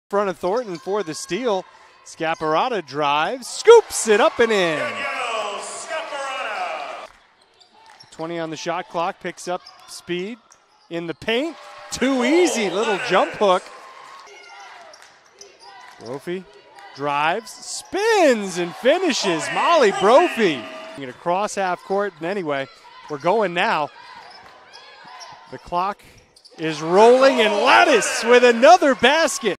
Women's Basketball Upsets No. 13 Stonehill (11/20/21)
The Saint Michael's women's basketball team upset 13th-ranked Stonehill, 75-72, on Nov. 20, 2021, behind senior Nicole Ladas' career-high 27 points.